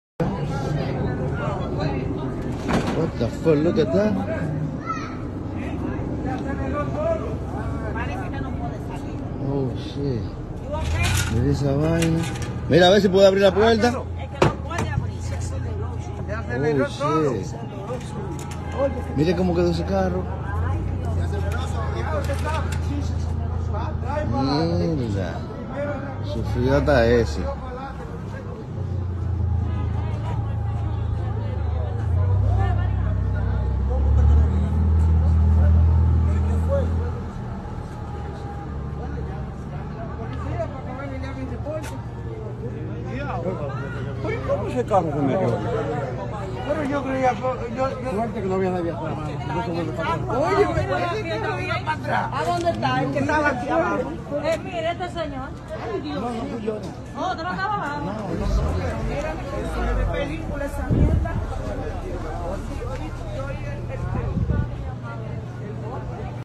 Street parking in New York City (Sickles St)
Someone could not find space to park his car so he improvised.